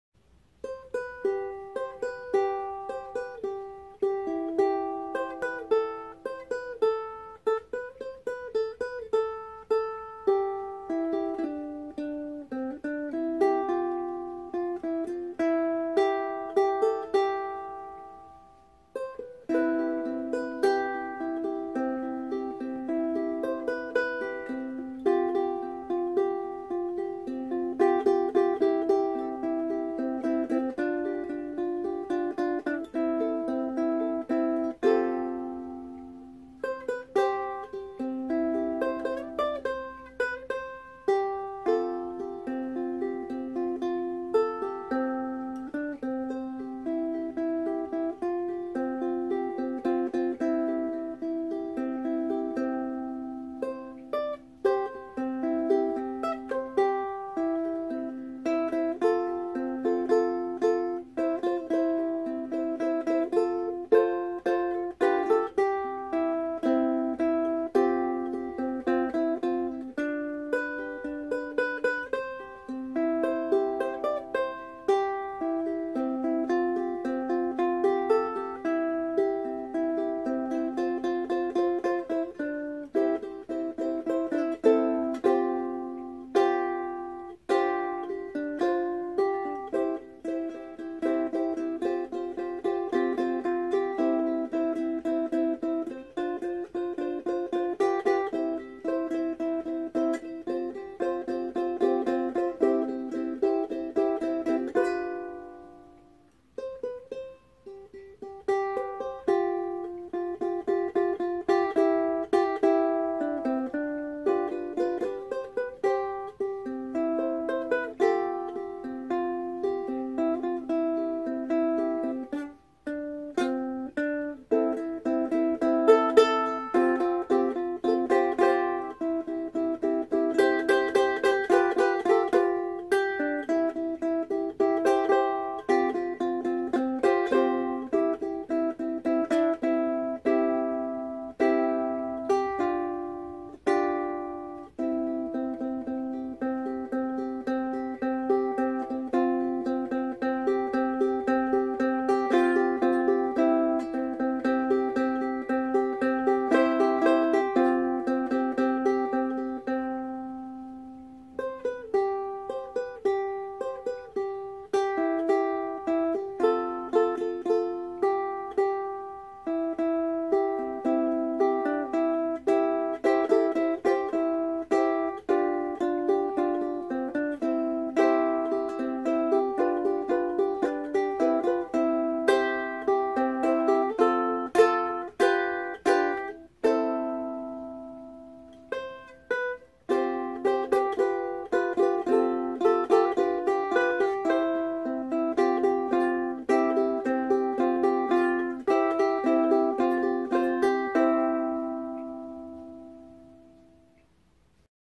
Improv 604: In Space, No One Can Hear Your Ukulele (Oscar Schmidt concert ukulele)
When I have a migraine, my titles get silly. I can't believe I played that whole piece with the lights on.